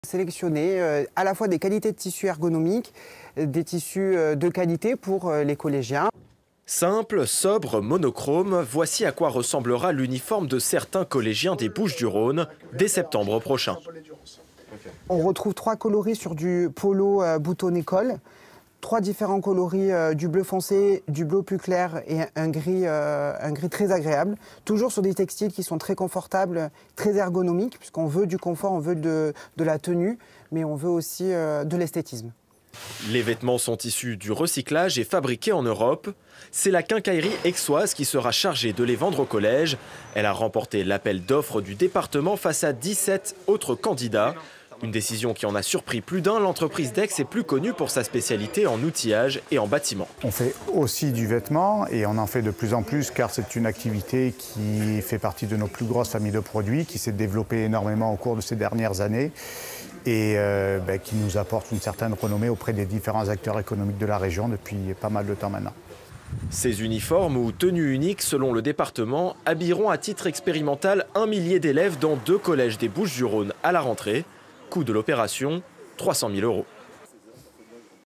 Uniforme pour les collégiens, la quincaillerie aixoise remporte l'appel d'offre
Une entreprise aixoise a remporté l'appel d'offre du département afin de produire les uniformes distribués dans les établissements participants.

Un reportage de : J. Boudier, N. Chaix-Bryan et E.Guez